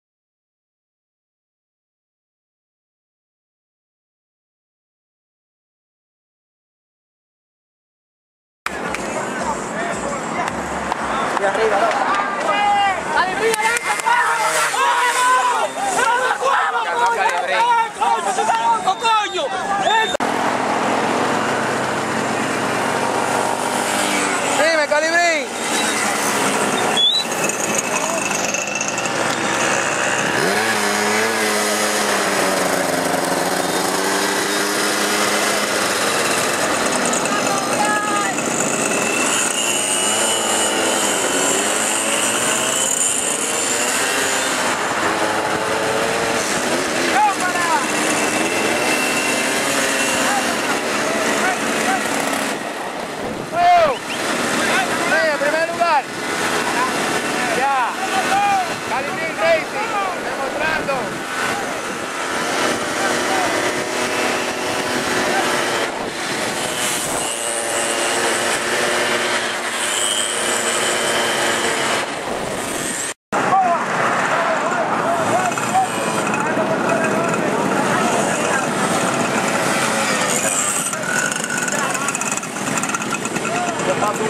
Lloron racing y calibrin le ganan a wilo y a barry bon
esta carrera fue el domingo 4 de julio en la pista nueva,osea en la 6 de noviembre.ganador: lloron racing y calibrin